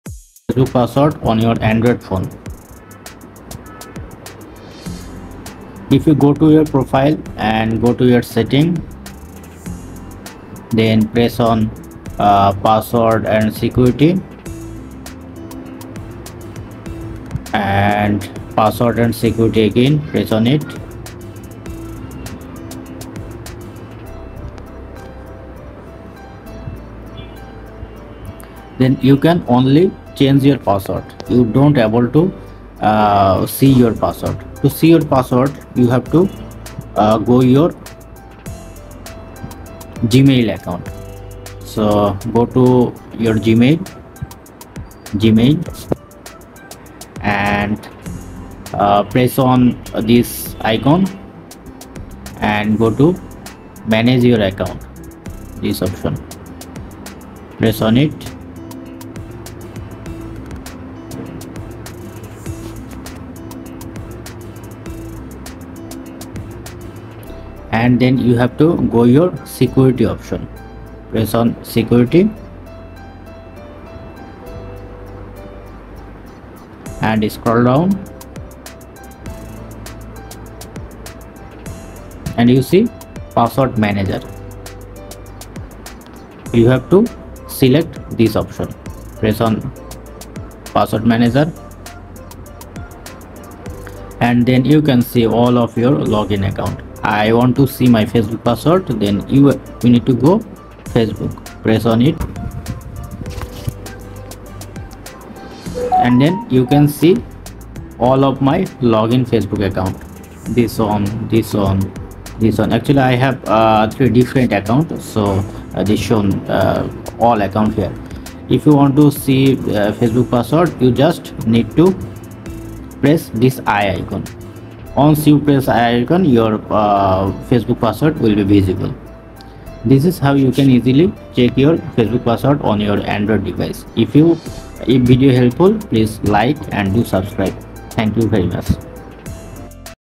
How to check Facebook password on Android 2024 - See my facebook password
How to check Facebook password on Android, Want to view saved Facebook password on an Android phone but don’t know exactly how to make it? This guide will show you how to see your Facebook password on Android with detailed steps.

0:00 Intro
0:10 Fb password and security setting
0:30 Change fb password
0:42 Go Gmail account
1:23 Password manager setting
 1:45 See Facebook password
2:10 Outro